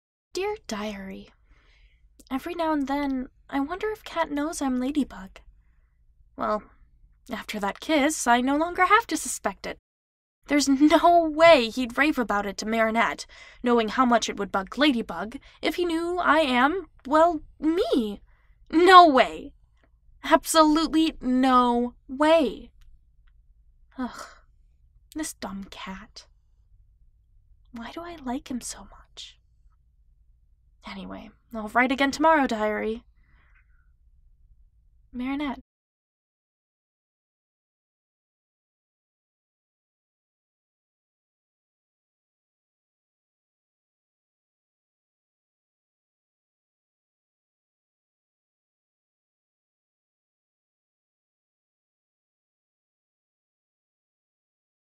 Dear Diary - Month 10, Day 20 (A Miraculous Ladybug Fanfiction)
Marinette writes in her diary - A Miraculous Ladybug fanfiction read aloud.

Read by Eleanor Rose